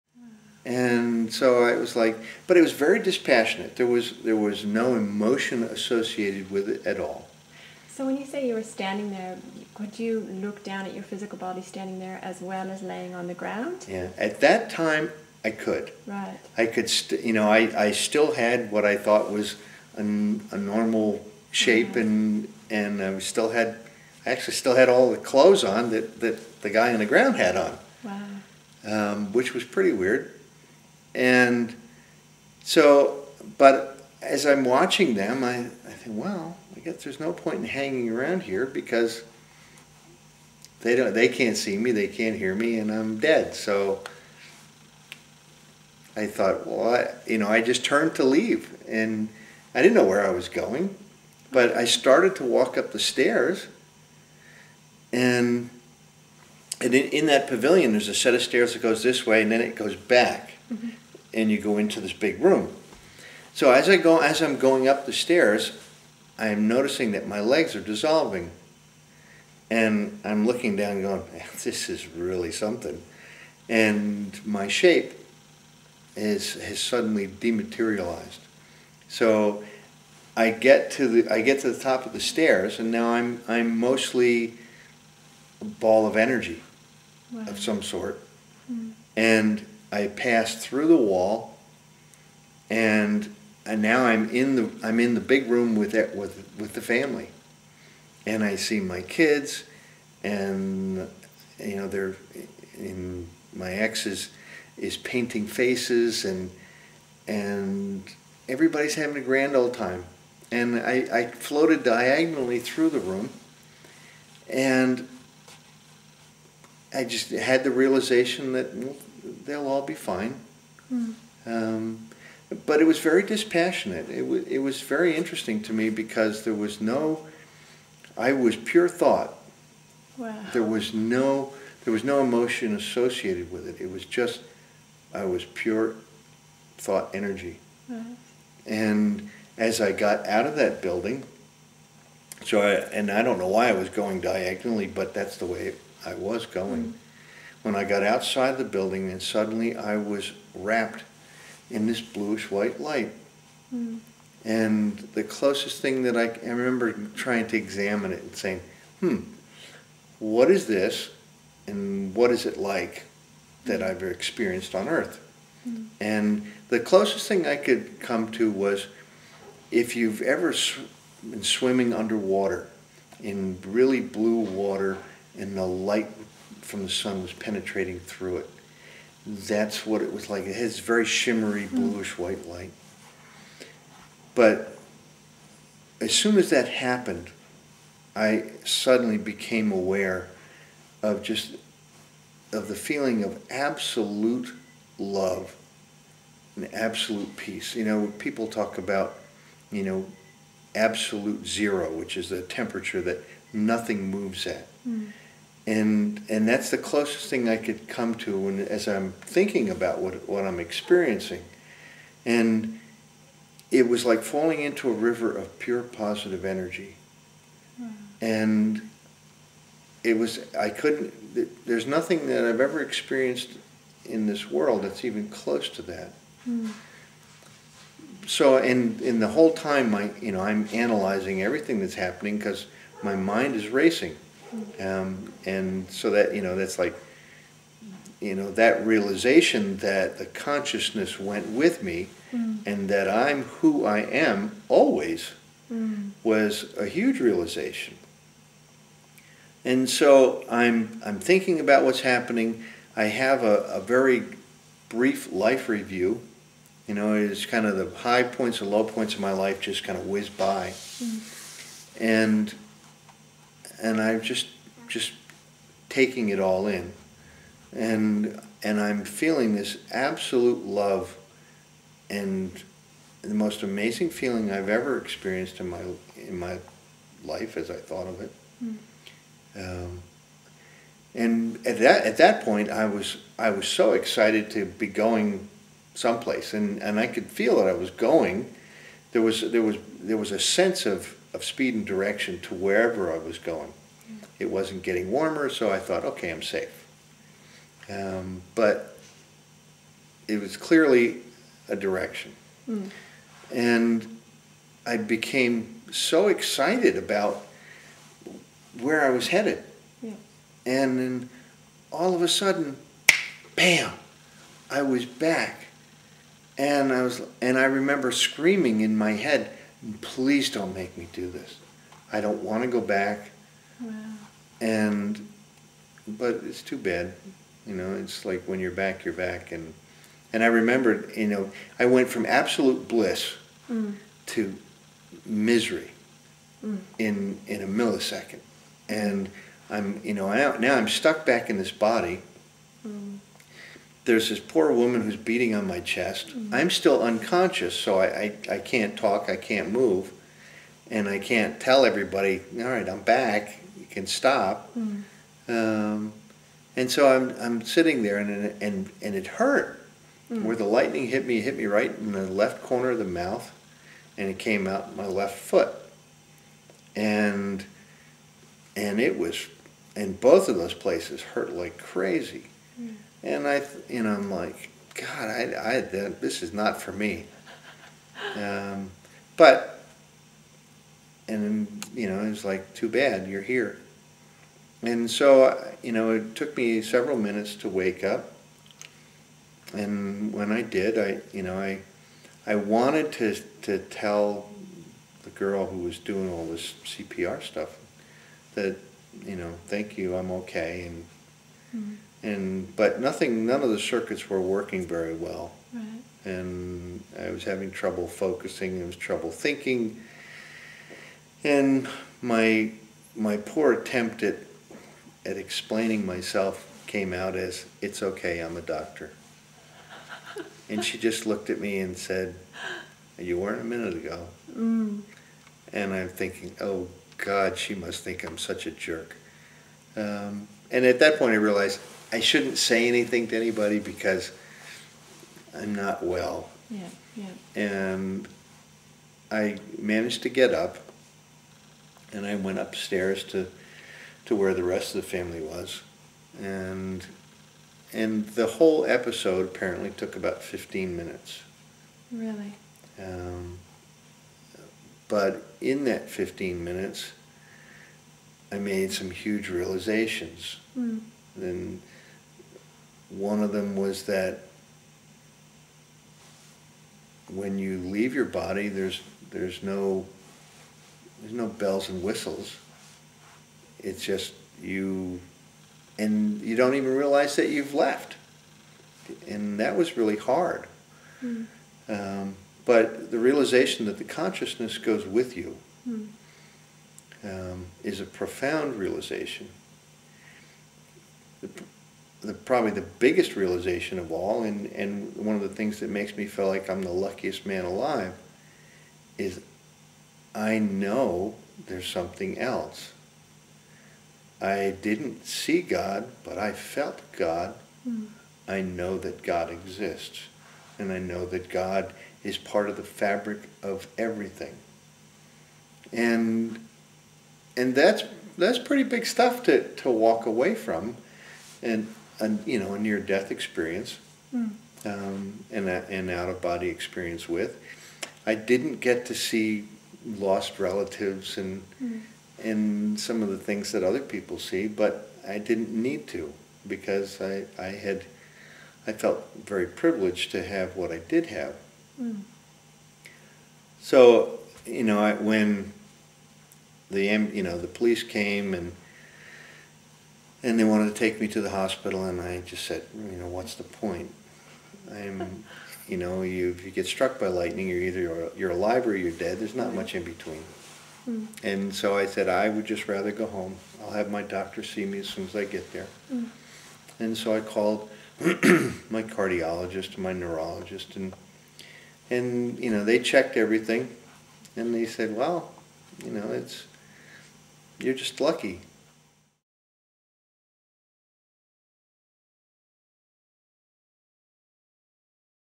Part 2 - Tony Cicoria - ITTV Show
After being struck by lightning and having a near-death experience, Dr Tony Cicoria began receiving what he terms "downloads" in the form of music composition for the piano.  Dr Cicorias incredible journey has proved to him that there is more to life than what we see.